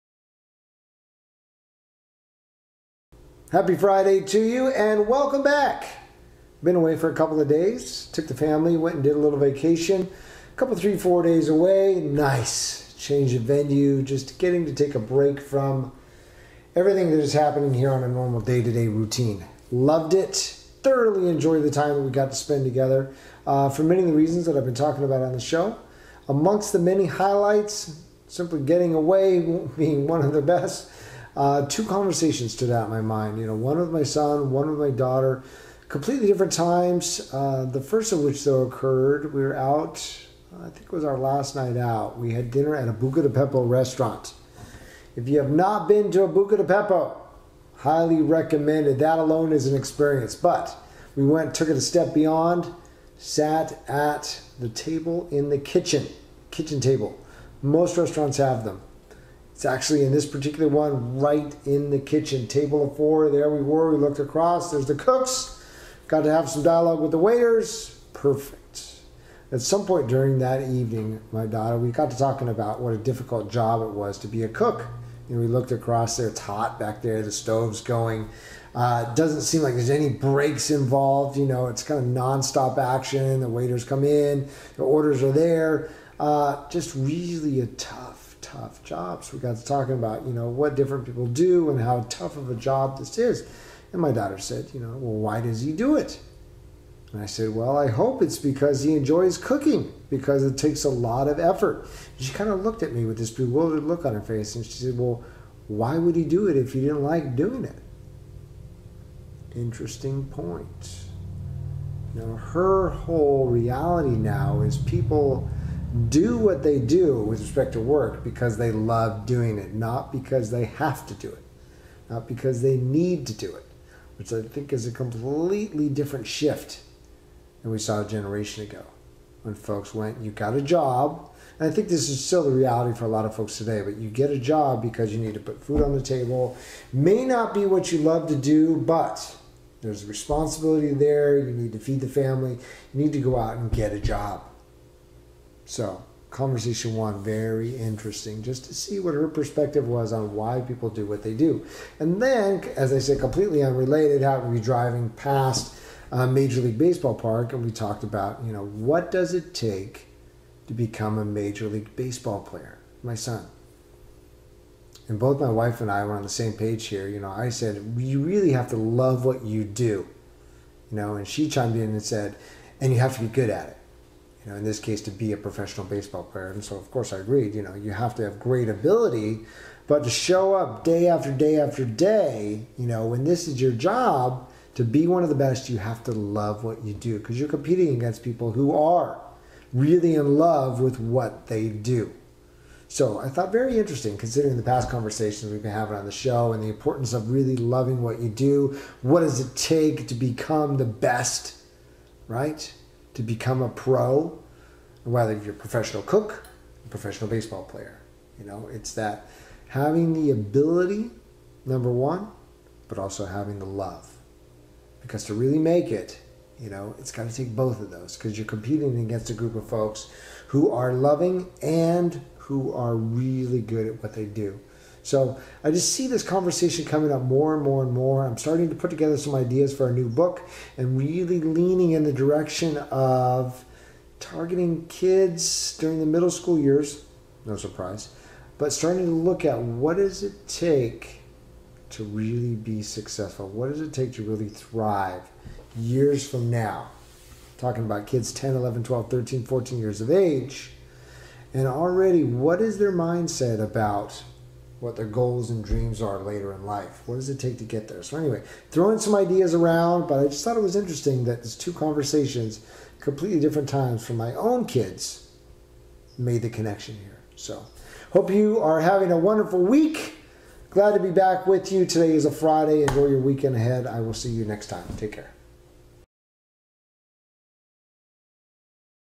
Show #196 - Ask your child, "What does it take to be successful?"
I'm curious. Today's show focuses on the key elements needed to become successful in adulthood as seen from the minds of kids today.